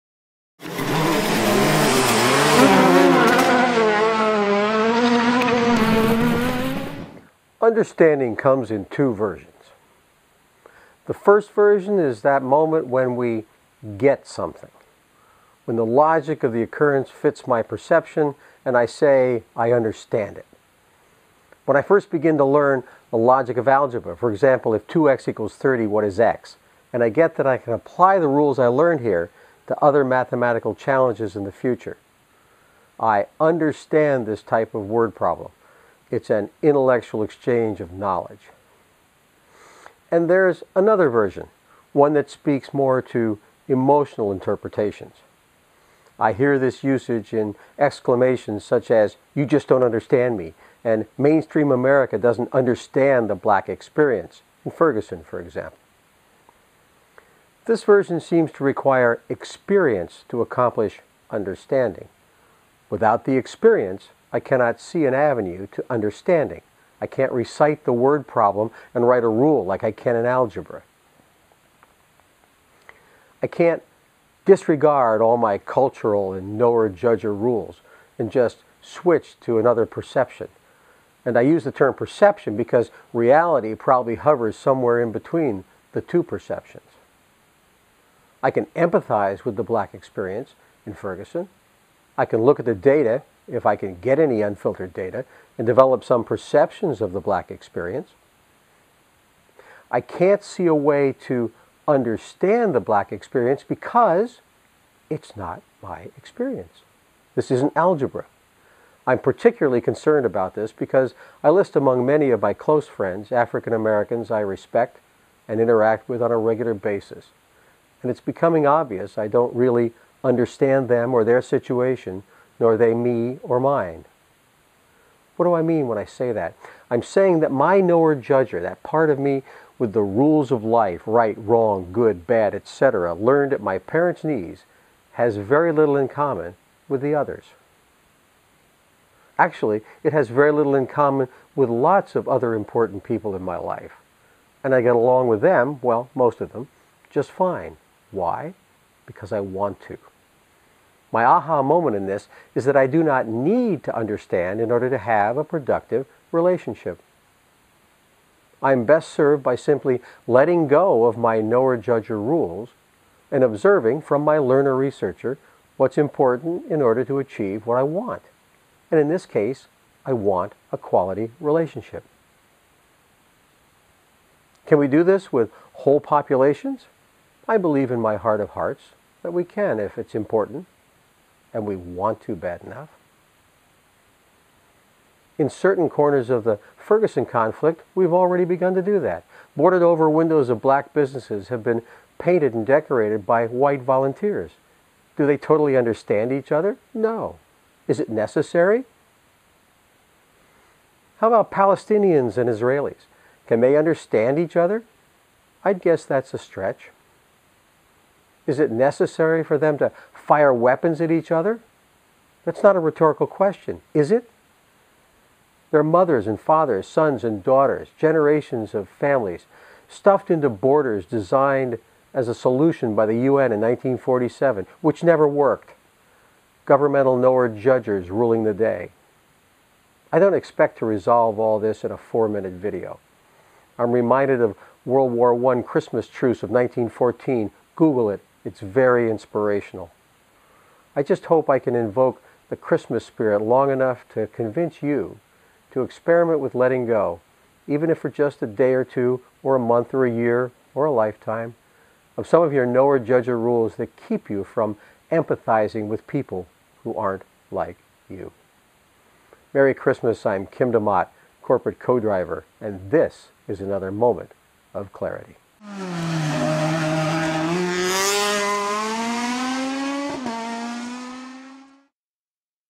Moment of Clarity, December, 2014...Do I HAVE to understand?
Steven Covey told us first understand, then be understood. I'm not so sure all that's necessary. If I have agendas it is. If I'm selling something it is. It forces us to concede value to Knower/Judger rules that can be quite dysfunctional. True connections (sometimes called intimate connections) avoid all that and put rules aside in order to form a bond. Christmas Truce of 1914 is one fine example 100 years ago on a gruesome battlefield. We can do this today. If we WANT to.